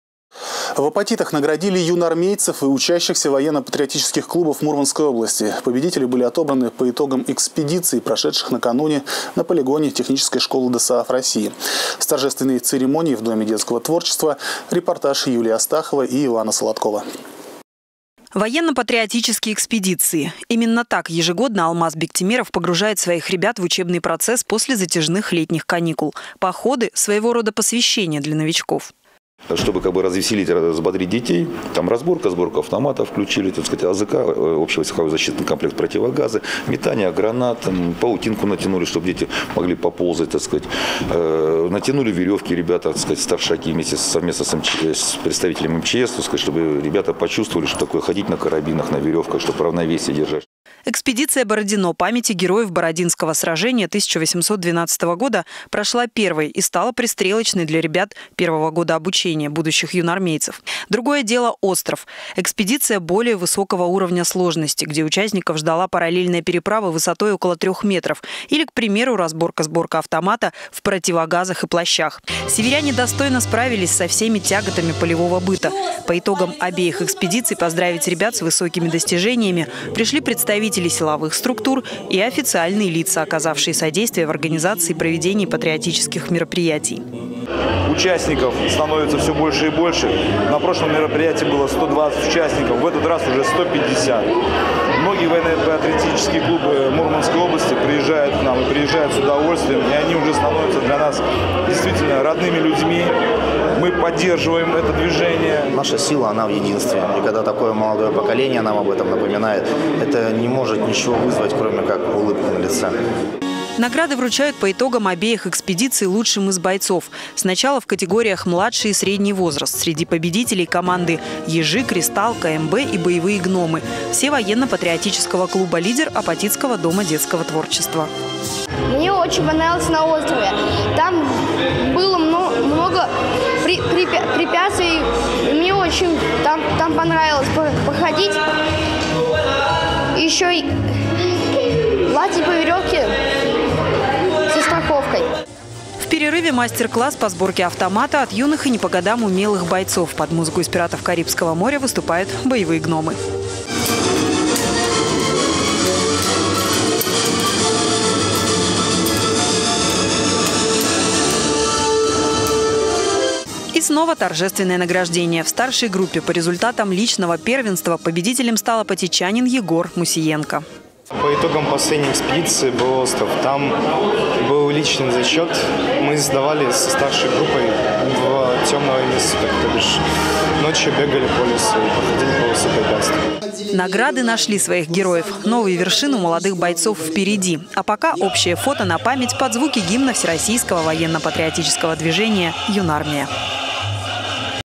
В Апатитах наградили юнармейцев
В Апатитах наградили юнармейцев и учащихся военно-патриотических клубов Мурманской области. Победители были отобраны по итогам экспедиций, прошедших накануне на полигоне технической школы ДОСААФ России. С торжественной церемонии в Доме детского творчества – репортаж  Юлии Астаховой и Ивана Солодкова.